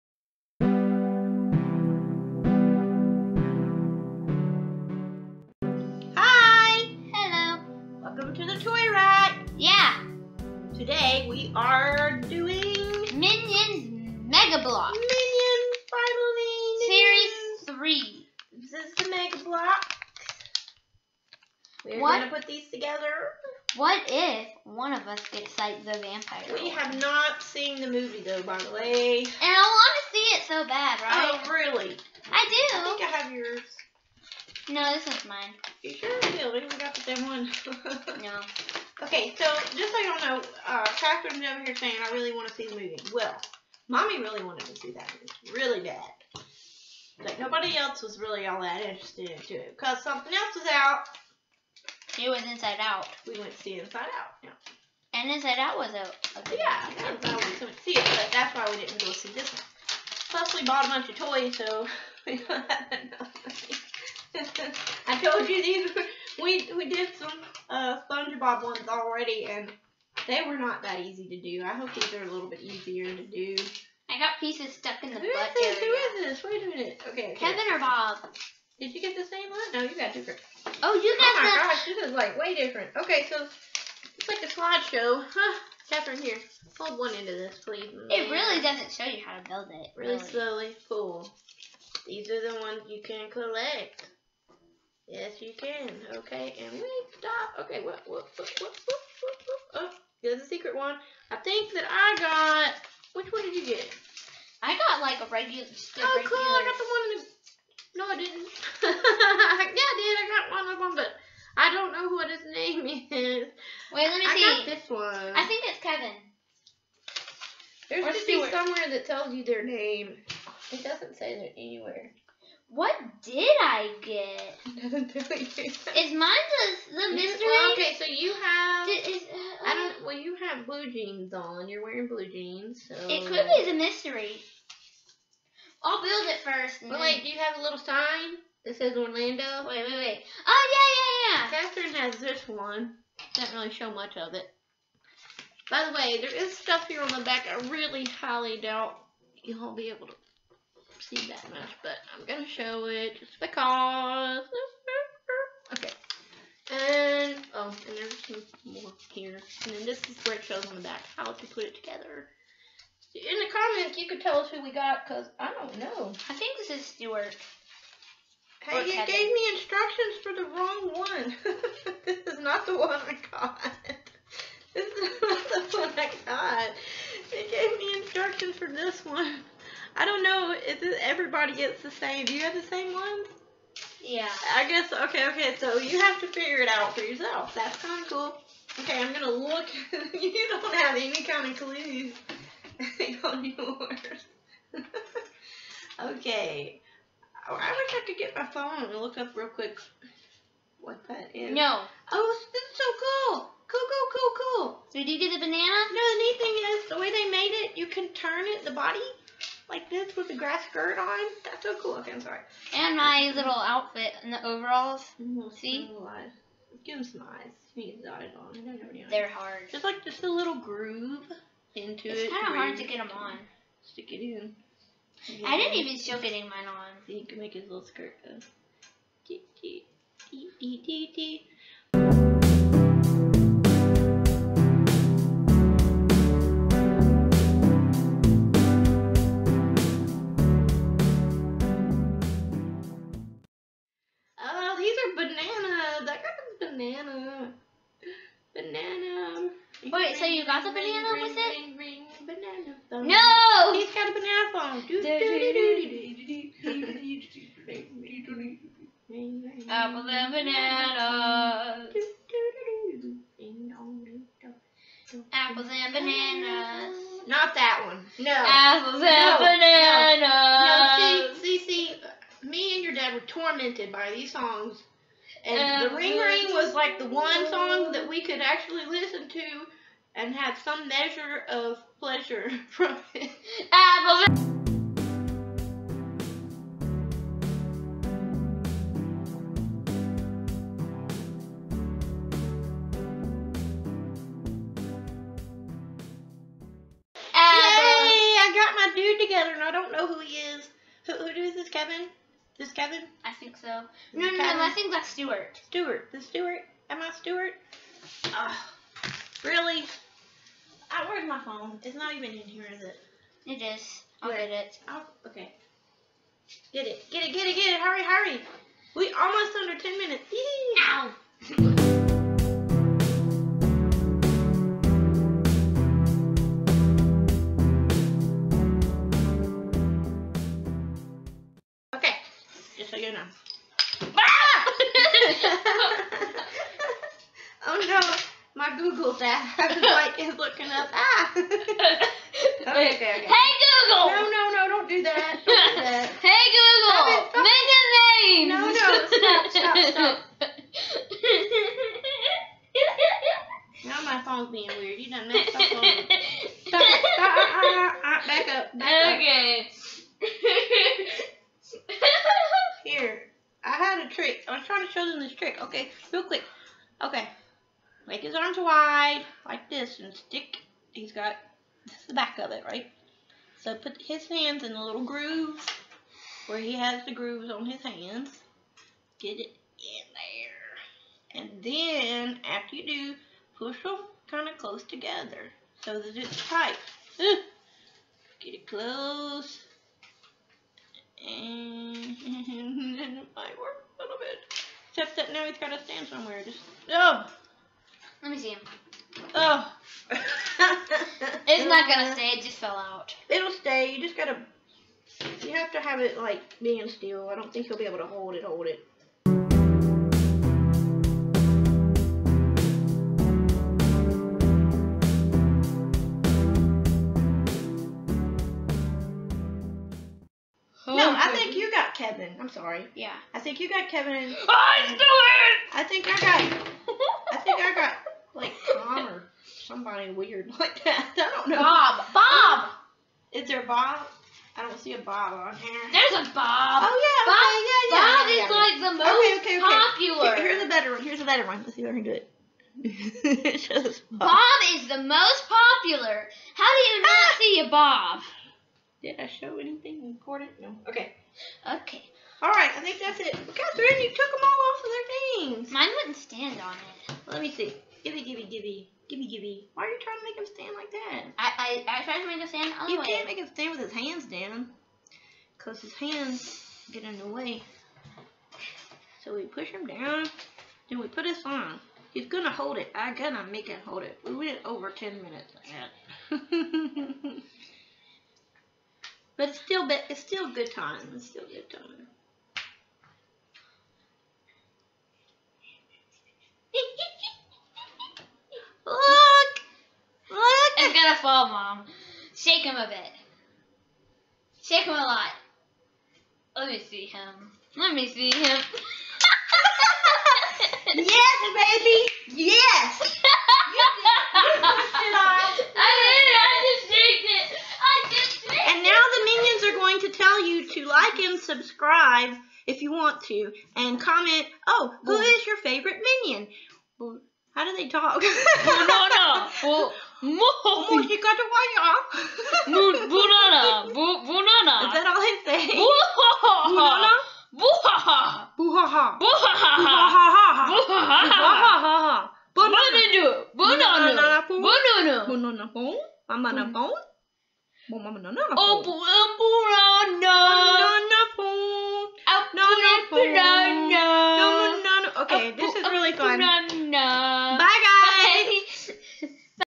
Minions Mega Bloks Mystery Figures!! Another Attempt at Movie Mega Bloks Blind Bags
Kat and I have fun putting some Minion Mini Figures from the Minion Movie by Mega Bloks together!! We had a lot of fun and laughs doing this. We just might make a builder out of Kat!!! We aren't 100% sure of which minions we got. Do you know?? Who is your fave minion?

Check out our instagram for past, current and future videos and candid pics of the family. Search

If you liked it, that makes us so happy. Though we hope you enjoyed it as much as we did.